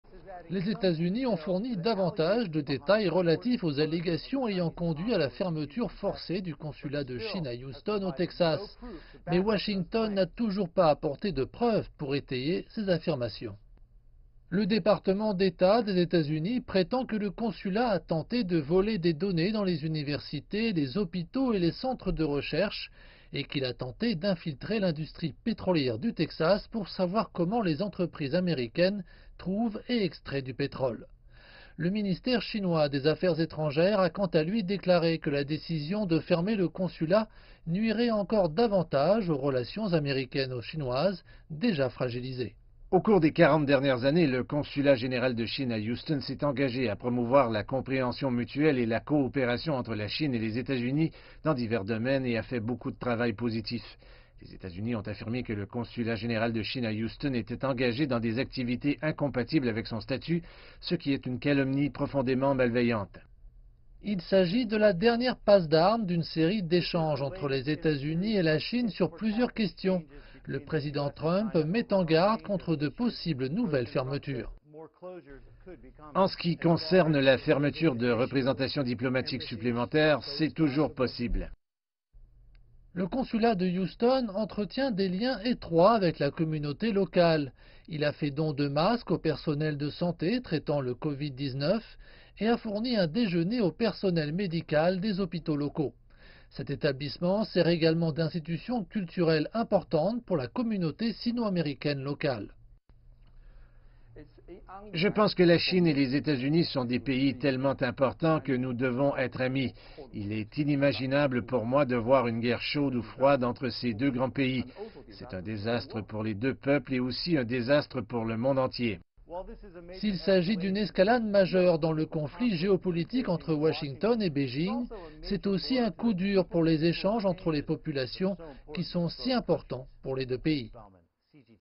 La Chine condamne les attaques américaines contre des diplomates et des chercheurs chinois
Les tensions entre la Chine et les États-Unis s'intensifient après la décision de Washington de fermer le consulat de Chine à Houston. Beijing condamne les actions américaines contre ses scientifiques et ses consulats.